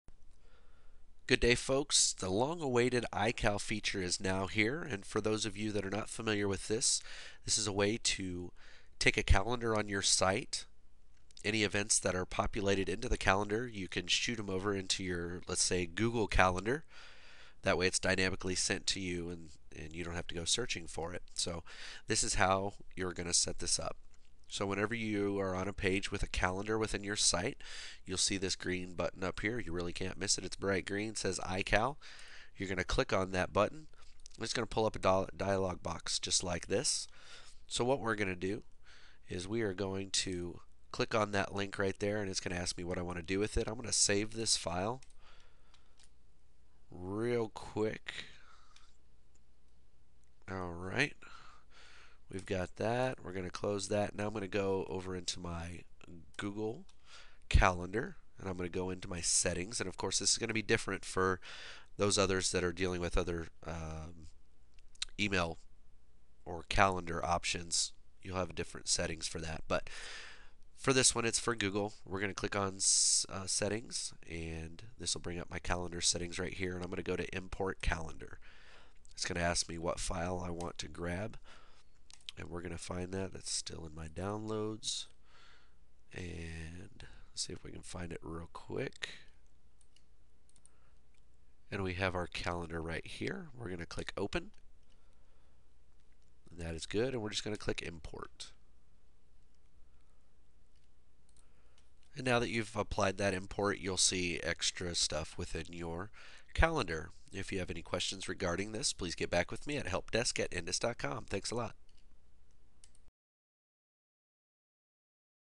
ICal Feeds
Use the new ical feed provided with all of your events to publish items to your desktop, calendar program, or smartphone.